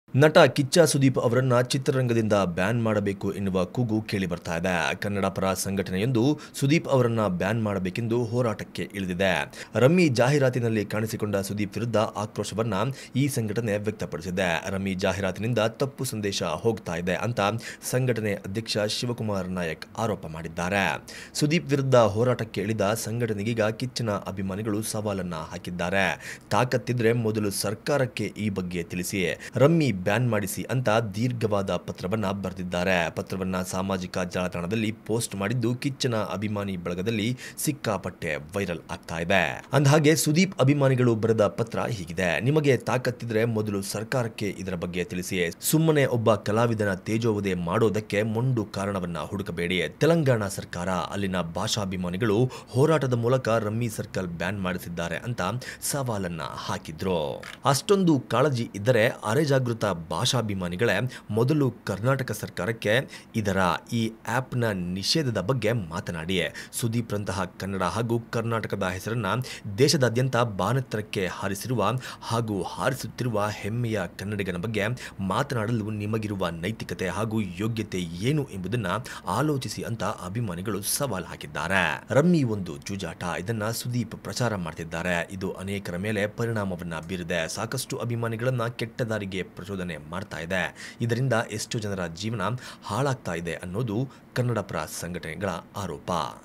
ಸುದೀಪ್ ಬ್ಯಾನ್ ಮಾಡಿ ಅಂದೋರ ವಿರುದ್ಧ ತಿರುಗಿಬಿದ್ದ ಅಭಿಮಾನಿಗಳು | Sudeep | Fans | Filmibeat Kannada
ನಟ ಕಿಚ್ಚ ಸುದೀಪ್ ರನ್ನು ಚಿತ್ರರಂಗದಿಂದ ಬ್ಯಾನ್ ಮಾಡಬೇಕು ಎನ್ನುವ ಕೂಗು ಕೇಳಿ ಬರುತ್ತಿದೆ. ಕನ್ನಡ ಪರ ಸಂಘಟನೆವೊಂದು ಸುದೀಪ್ ಅವರನ್ನು ಬ್ಯಾನ್ ಮಾಡಬೇಕೆಂದು ಹೋರಾಟಕ್ಕೆ ಇಳಿದಿದೆ. ರಮ್ಮಿ ಜಾಹಿರಾತಿನಲ್ಲಿ ಕಾಣಿಸಿಕೊಂಡ ಸುದೀಪ್ ವಿರುದ್ಧ ಆಕ್ರೋಶ ವ್ಯಕ್ತಪಡಿಸುತ್ತಿದ್ದಾರೆ. ರಮ್ಮಿ ಜಾಹಿರಾತಿನಿಂದ ತಪ್ಪು ಸಂದೇಶ ಹೋಗುತ್ತಿದೆ ಎಂದು ಸಂಘಟನೆ ಅಧ್ಯಕ್ಷ ಶಿವಕುಮಾರ್ ನಾಯಕ್ ಆರೋಪ ಮಾಡಿದ್ದಾರೆ.

Sudeep Fans challenge to Kannada Activists for ban rummycircle. Kannada Activists Filed A Complaint In Film Chamber To Ban Sudeep.